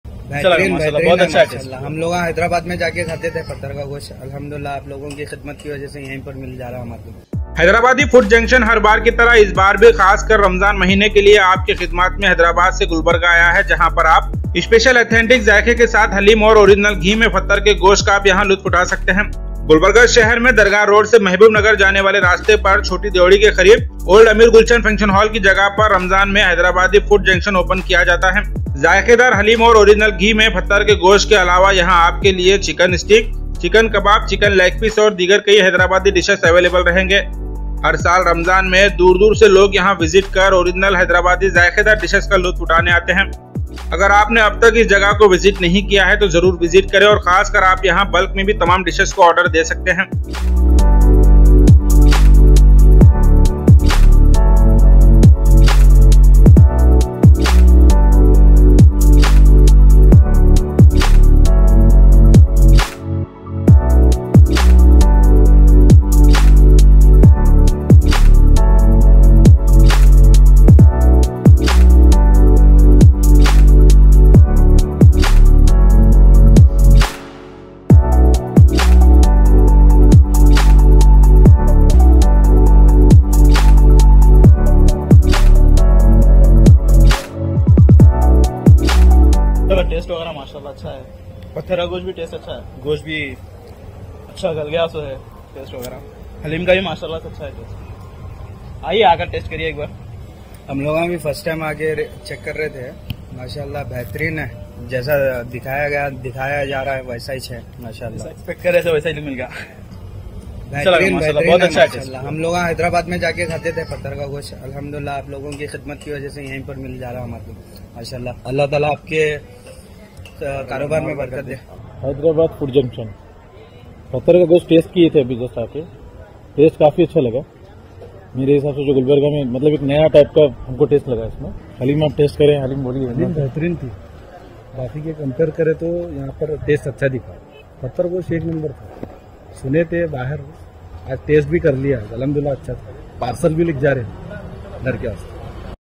Spl Hyderabadi delicacies like Haleem and Pathar Ka Gosht, available at Hyderabadi Food Junction.
Spl Hyderabadi delicacies like  Haleem and Pathar Ka Gosht, available at Hyderabadi Food Junction near Choti Deodi Dargah on the Mehboob Nagar Road in Gulbarga.